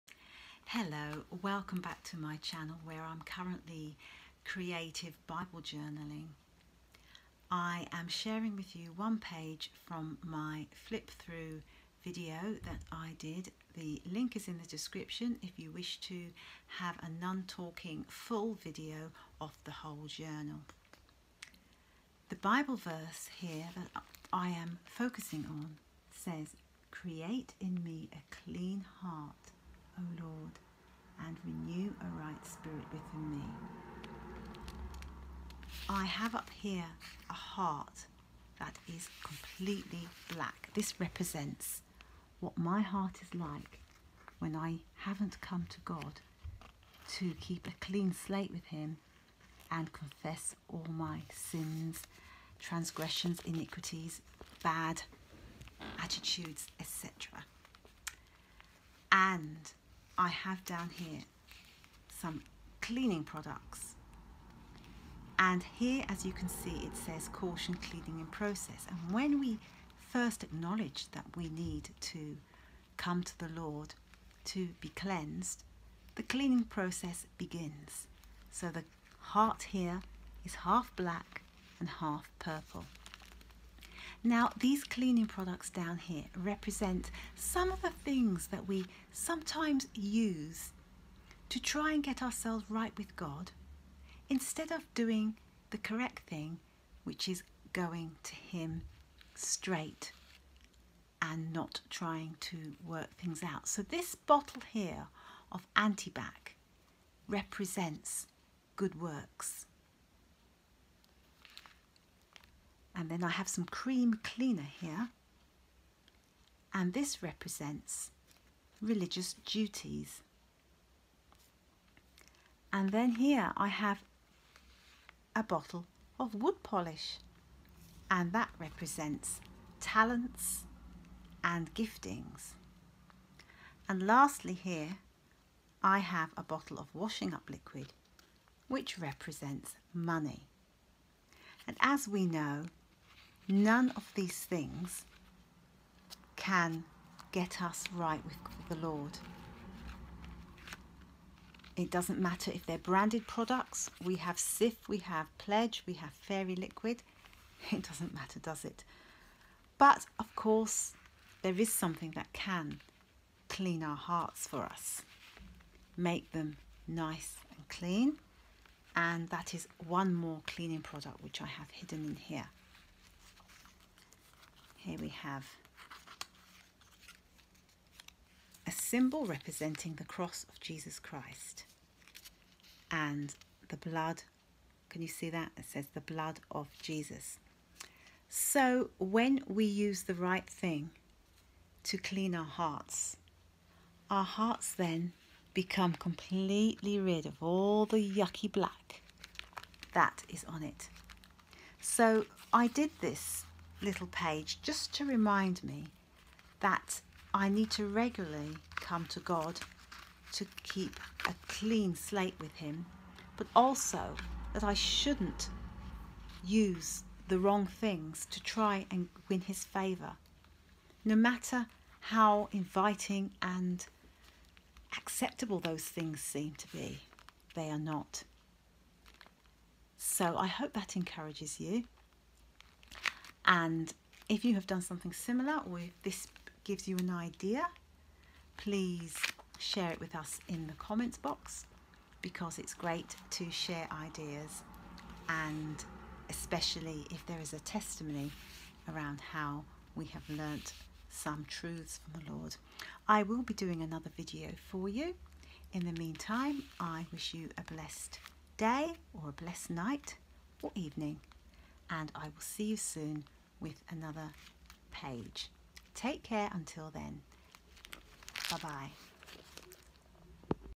Creative bible journaling | Psalm 51 | Creative in me a clean heart.
- Here's a creative journaling page featuring the bible verse from Psalm 51, 'Creative in me a clean heart oh Lord...'